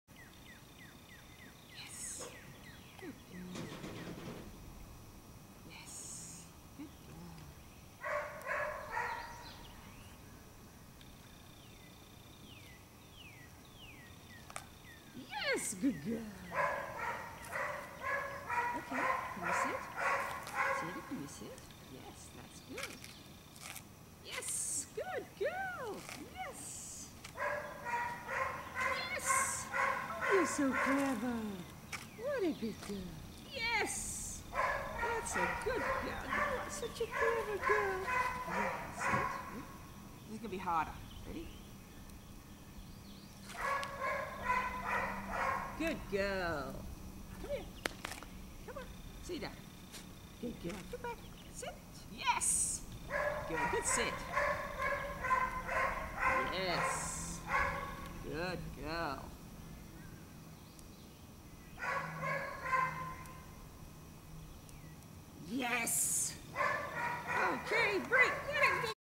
K2PDF Adv Rule Outs
K2PDF 1 - Adv. Rule Outs.

Cedar on the driveway.  The treats are kibble and you will have to use your imagination as to where they fall!